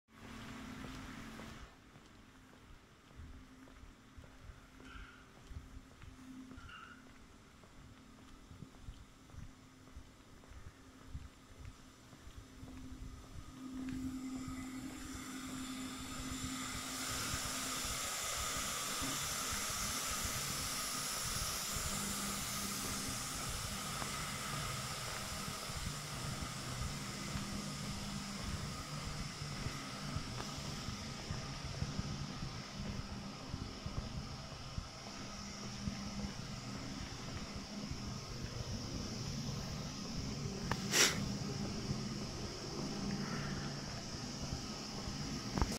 Tipogrāfija ziemassvētkos
Slēgta ražošana Ziemassvētku brīvdienās